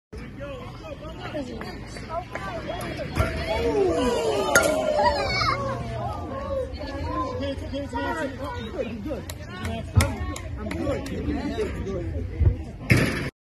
A un niño se le resbala el bate e impacta en el cráneo de su entrenador: el sonido es atroz | AS
Mientras un niño calentaba su swing abanicando al aire antes de jugar el bate se le resbaló y fue a parar deirecto a la cabeza de entrenador. Por suerte no fue grave.